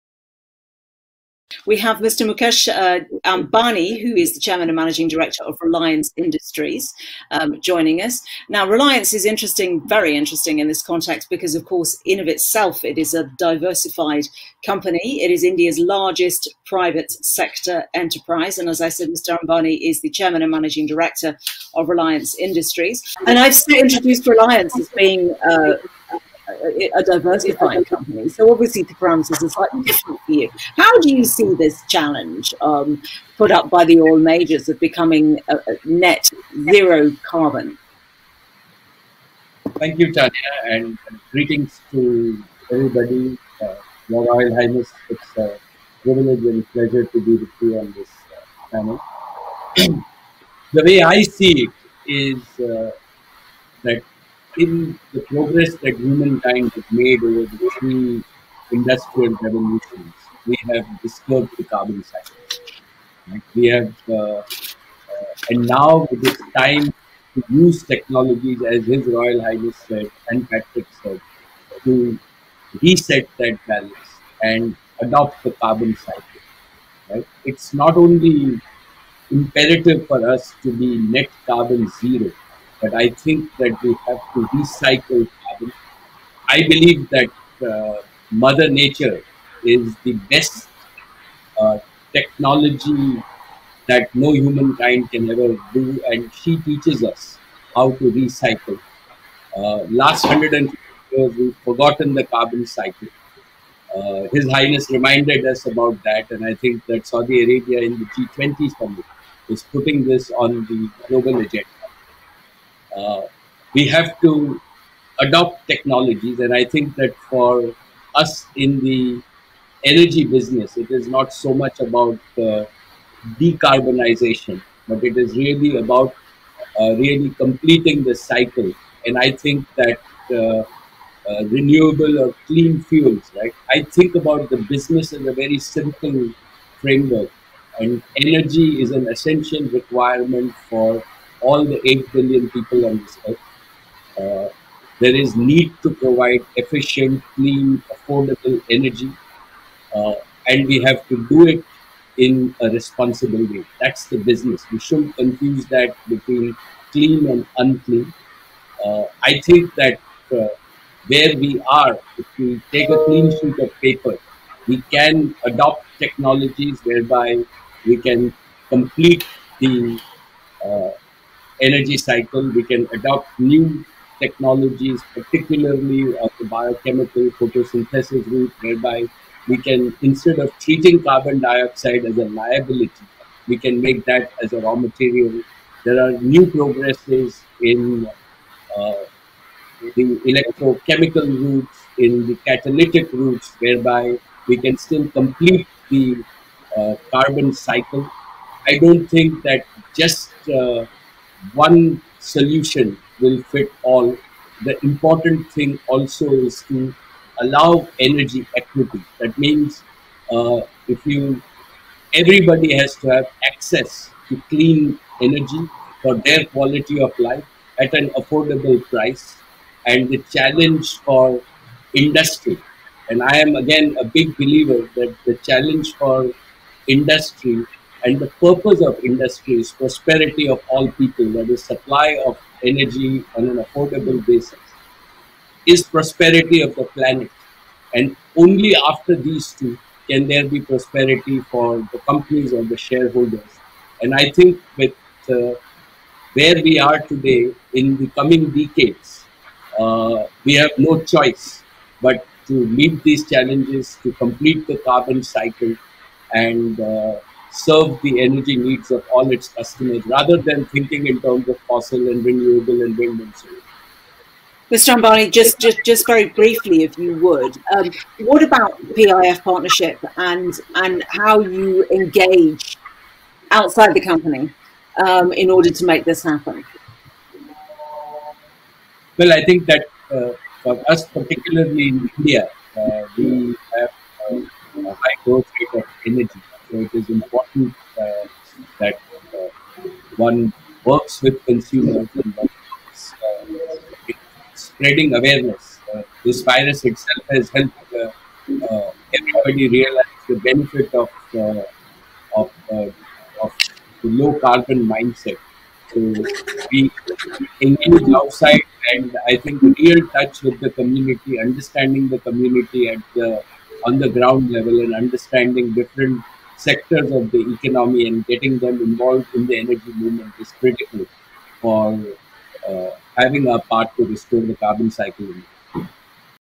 Mukesh Ambani pushes for clean, affordable energy; tech to decarbonise
New Delhi: In his biggest push for clean and affordable energy, billionaire Mukesh Ambani on Friday said it was imperative to adopt technologies that can recycle to set the carbon balance right.

 With Prince Abdulaziz bin Salman Al-Saud, Energy Minister of the world's largest oil exporter Saudi Arabia, listening, Ambani said there is a need to provide efficient, clean, and affordable energy.

 "The way I see is that in the progress that humankind has made (between) industrial revolutions, we have disturbed the carbon cycle and now it is time to use technology... to reset that balance and adopt the carbon cycle right," he said speaking at the FII Investment Institute Conference over a video link.

 He went on to stress the need for technologies that can recycle carbon dioxide.

 Statement by the Chairman and Managing Director of Reliance Industries Ltd, India's most valuable company, is significant considering that his firm operates the world's biggest oil refining complex at Jamnagar in Gujarat.

 "I think that, where we are, if we take a clean sheet of paper and adopt technologies whereby we can complete the energy cycle, we can adopt new technologies particularly biochemical photosynthesis. Instead of treating carbon dioxide as a liability - we can make that as raw material," he said.

"It's not only imperative for us to be net carbon zero but I think that we should opt to recycle carbon."

 Ambani said for those in the energy business, it is not so much about the decarbonisation but about completing the cycle for zero carbon emission.

 "Energy is an essential requirement for all 8 billion people on this earth. There is a need to provide efficient, clean, affordable energy. And we have to do it in a responsible way. That's the business. We should not confuse that between clean and unclean," he said.

 The oil-to-telecom conglomerate head said no one solution will fit all.

 "The important thing also is to allow energy equally. That means, everybody has to have access to clean energy for their quality of life at an affordable price," he said.

 Ambani's company buys a significant quantity of crude oil from Saudi Arabia for processing at its twin refinery complex at Jamnagar.

 It is in talks to sell a fifth of its oil-to-chemical business to Saudi Aramco for an asking of USD 15 billion.

"The new supply of energy on an affordable basis is the prosperity of the planet. And only after these two, there can be prosperity for the companies and the shareholders, and I think with where we are today, in the coming decades we have no choice, but to meet these challenges to complete the carbon cycle.

"And serve the energy needs of all its customers rather than thinking in terms of fossil and renewable and wind and so forth," he added.

Saudi sovereign wealth fund PIF has picked up a 2.3 per cent stake in Ambani's digital arm Jio Platforms Ltd.

"For us, particularly in India, we have high growth so it is important that one works with consumers in spreading awareness. This virus itself has helped everybody realise the benefit of low carbon mindset," he said.

"Real touch with the community, understanding the community on the ground level and understanding different sectors of the economy and getting them involved in the energy movement is critical as for having a part of restoring the carbon cycle."